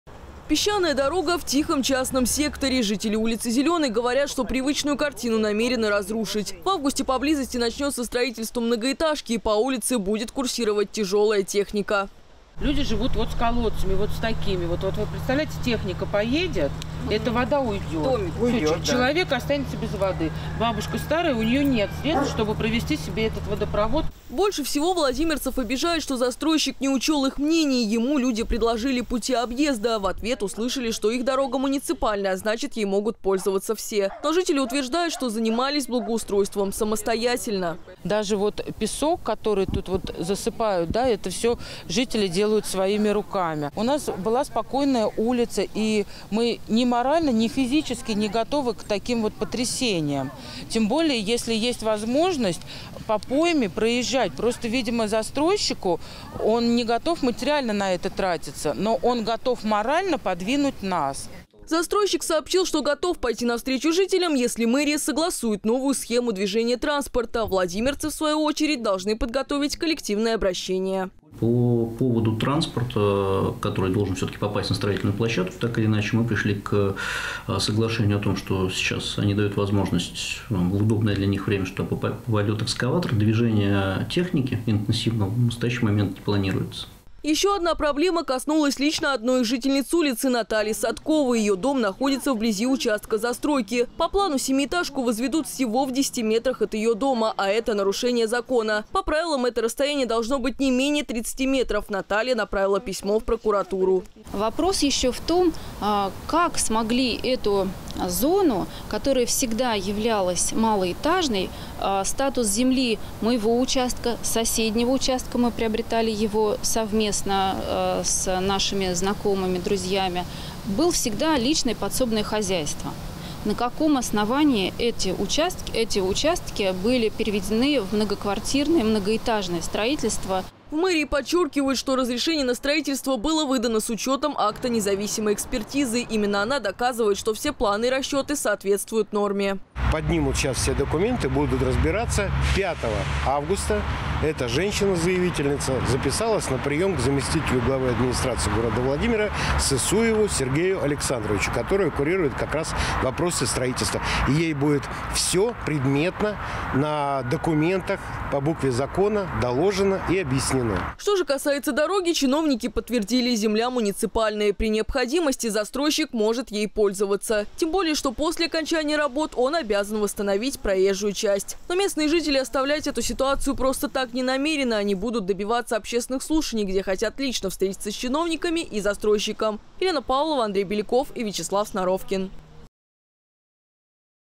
Жители Коммунара протестуют против спецтехники, которую хотят пустить по их дороге (2021 08 03)
Жители микрорайона Коммунар во Владимире пытаются решить проблему с застройщиком. Строительная компания планирует направить свою спецтехнику по одной из жилых улиц. Владимирцы обеспокоены, грузовые машины разобьют и без того плохую дорогу. Беспокоит местных жителей и слишком близкое расположение новостройки к частному дому.